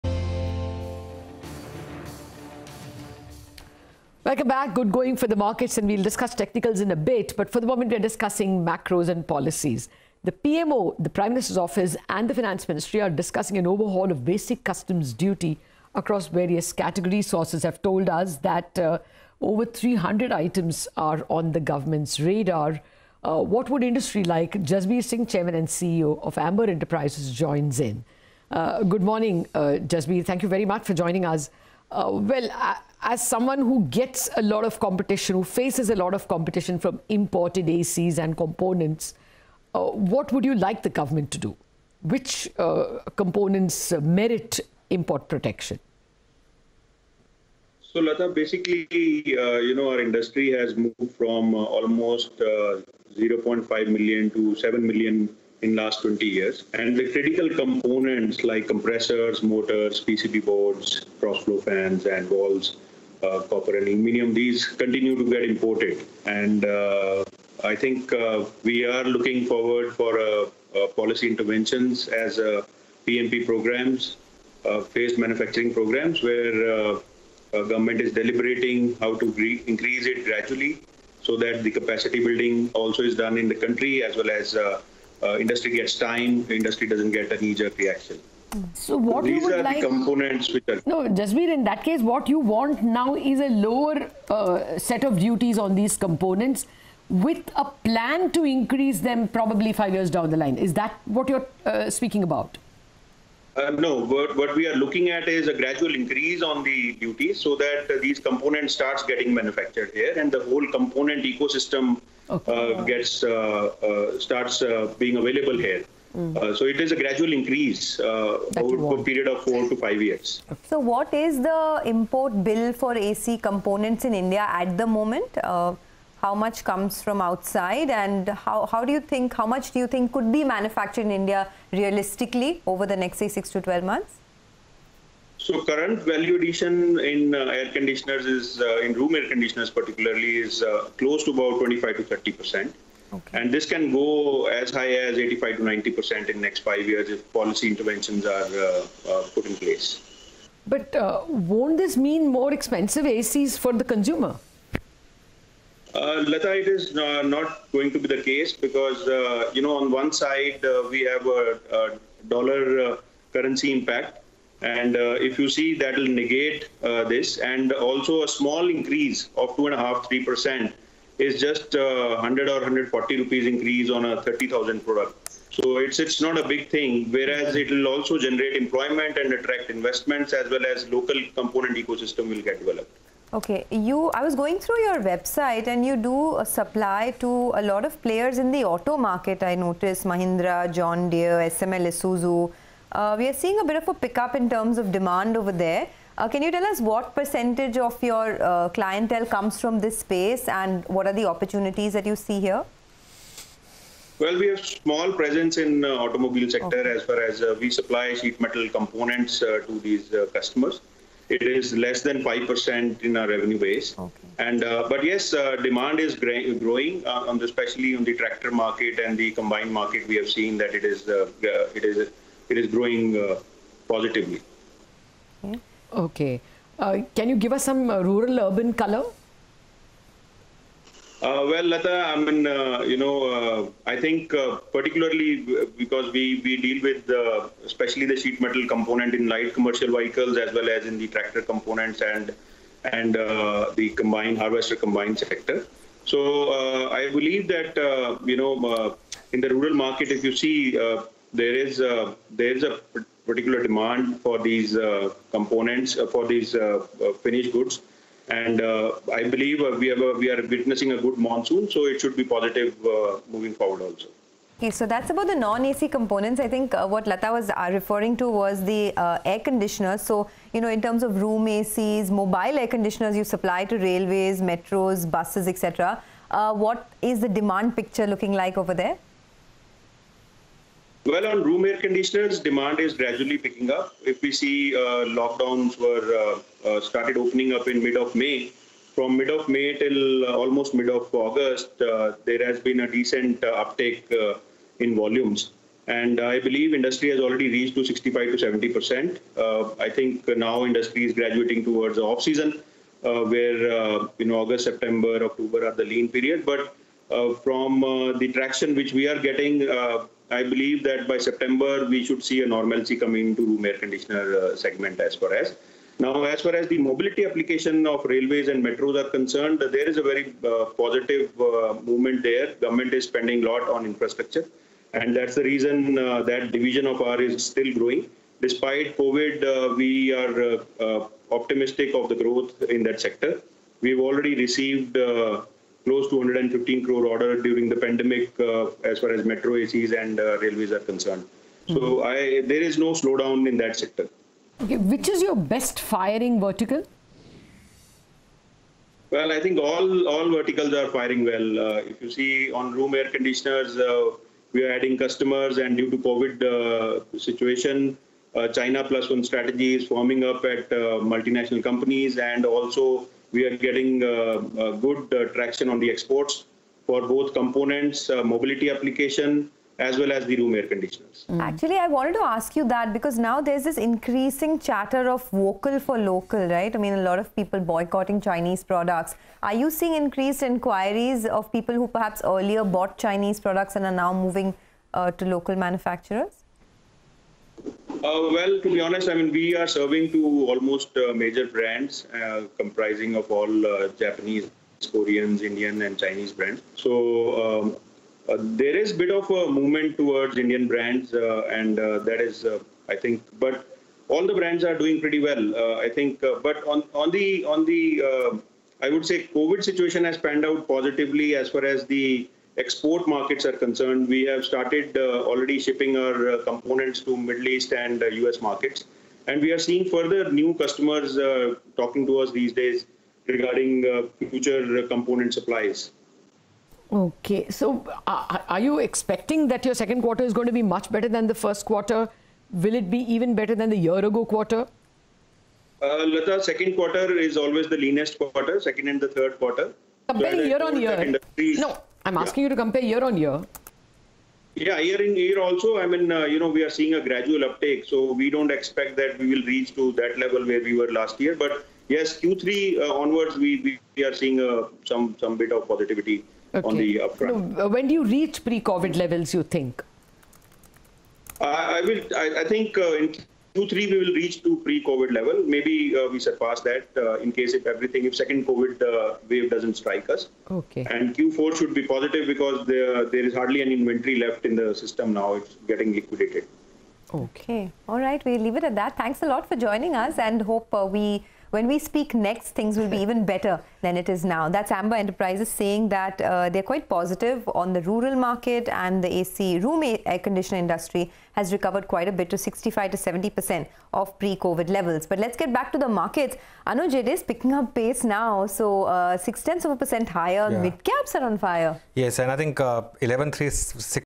Jasbir Singh's Views On Overhaul Of Custom Duties Likely This Week | Bazaar Corporate Radar
PMO & Finance Ministry are discussing fresh overhaul of basic custom duties this week, sources are telling us that over 300 items are on the radar. We caught up with Jasbir Singh of Amber Enterprise to discuss more.